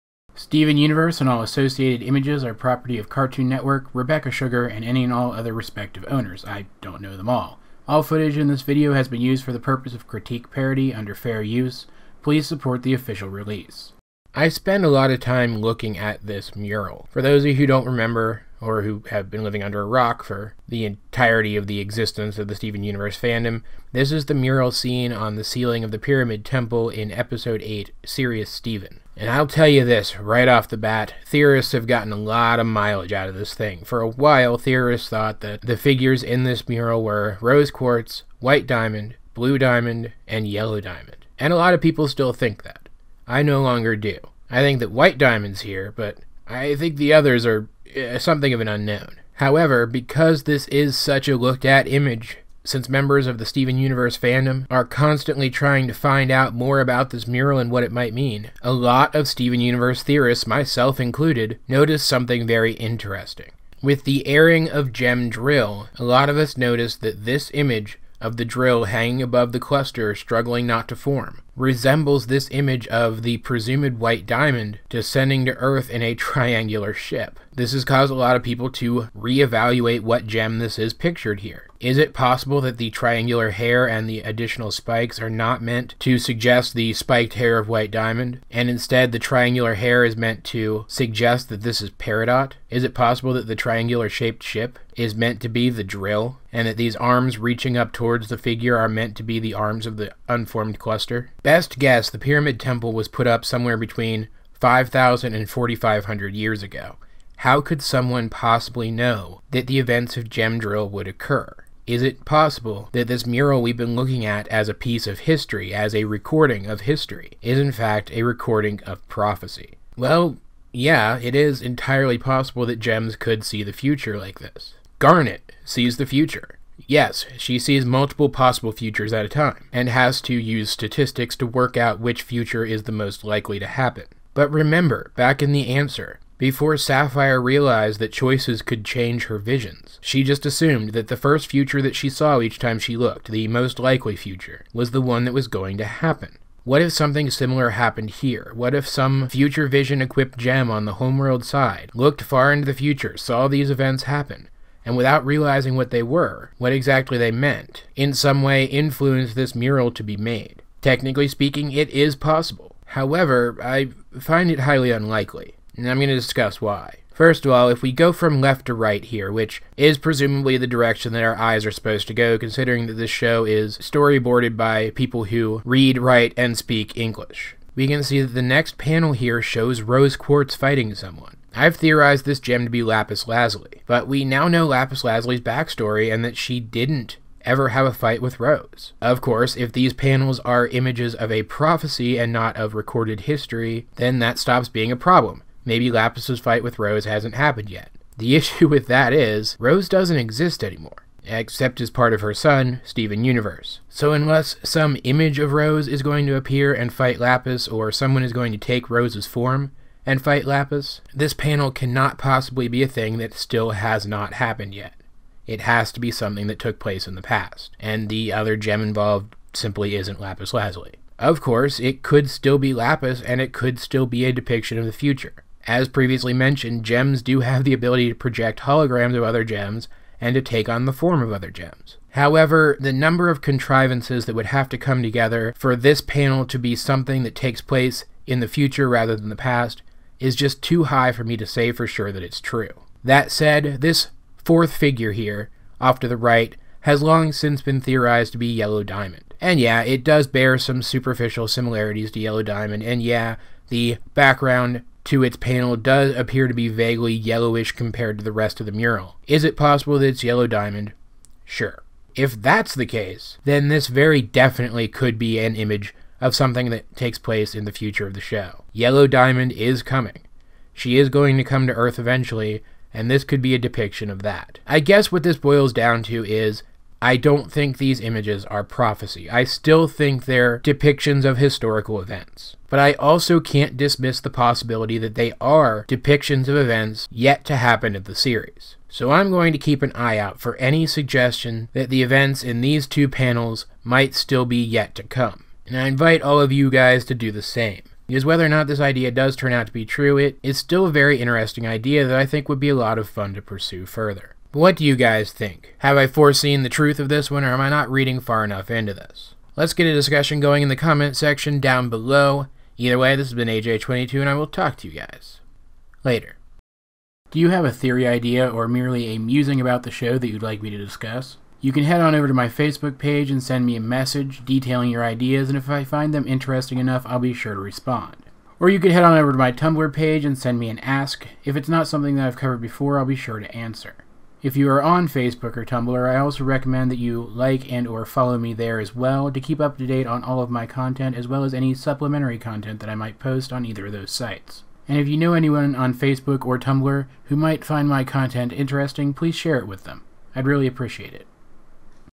Steven Universe Theory - History or Prophesy?
A look at the Pyramid Temple murals in the context of a recent episode.